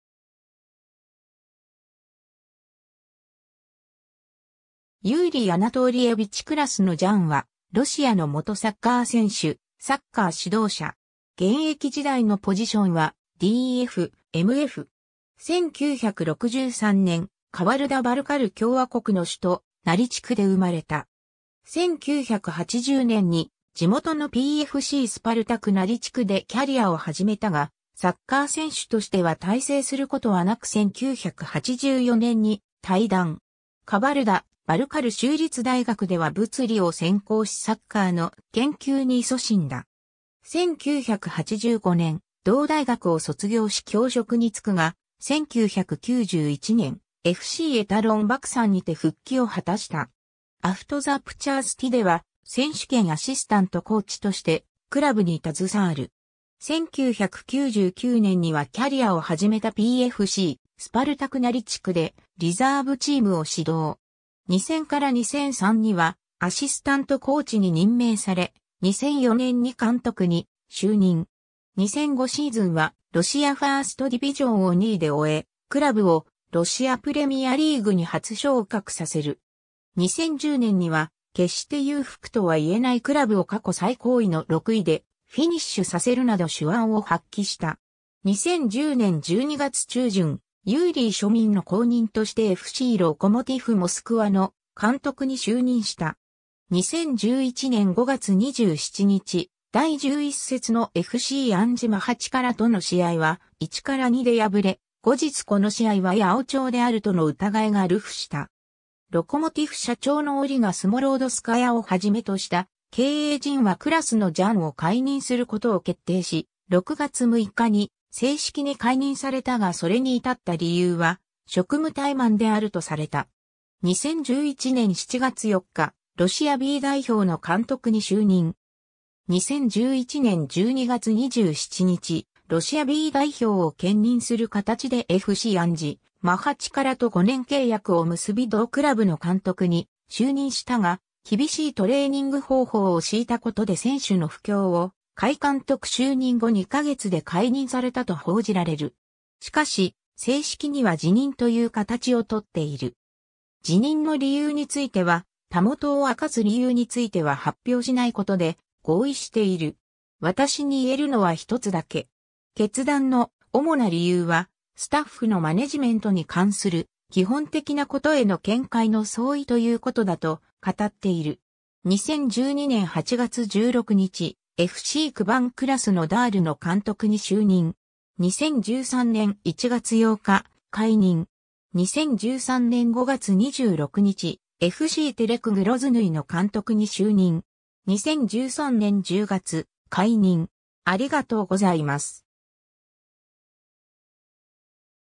ユーリー・クラスノジャン
PFCスパルタク・ナリチクの選手
ユーリー・アナトーリエヴィチ・クラスノジャン（Юрий Анатольевич Красножан、1963年6月7日 - ）は、ロシアの元サッカー選手、サッカー指導者。
現役時代のポジションはDF/MF。
 1963年、カバルダ・バルカル共和国の首都ナリチクで生まれた。
1980年に地元のPFCスパルタク・ナリチクでキャリアを始めたが、サッカー選手としては大成することは無く1984年に退団。
カバルダ・バルカル州立大学では物理を専攻しサッカーの研究に勤しんだ。
1985年、同大学を卒業し教職に就くが、1991年、FCエタロン・バクサン(後にFCアフトザプチャースティ・バクサンに改称)にて復帰を果たした。
アフトザプチャースティでは選手兼アシスタント・コーチとしてクラブに携わる。
1999年にはキャリアを始めたPFCスパルタク・ナリチクでリザーブチームを指導。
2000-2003にはアシスタント・コーチに任命され、2004年に監督に就任。
2005シーズンはロシア・ファーストディビジョン(実質2部)を2位で終え、クラブをロシア・プレミアリーグに初昇格させる。
2010年には決して裕福とはいえないクラブを過去最高位の6位でフィニッシュさせるなど手腕を発揮した。
2010年12月中旬、ユーリー・ショミンの後任としてFCロコモティフ・モスクワの監督に就任した。
 2011年5月27日、第11節のFCアンジ・マハチカラとの試合は1-2で敗れ、後日この試合は八百長であるとの疑いが流布した。
ロコモティフ社長のオリガ・スモロードスカヤをはじめとした経営陣はクラスノジャンを解任することを決定し、6月6日に正式に解任されたがそれに至った理由は「職務怠慢」であるとされた。
 2011年7月4日、ロシアB代表の監督に就任。
 2011年12月27日、ロシアB代表を兼任する形でFCアンジ・マハチカラと5年契約を結び同クラブの監督に就任したが、厳しいトレーニング方法をしいた事で選手の不興を買い監督就任後2ヶ月で解任されたと報じられる。
しかし、正式には辞任という形をとっている。
辞任の理由については「たもとを分かつ理由については発表しないことで合意している。私に言えるのは一つだけ。決断の主な理由は、スタッフのマネジメントに関する基本的なことへの見解の相違ということだ」と語っている。
 2012年8月16日、FCクバン・クラスノダールの監督に就任。
2013年1月8日、解任。
 2013年5月26日、FCテレク・グロズヌイの監督に就任。
2013年10月、解任。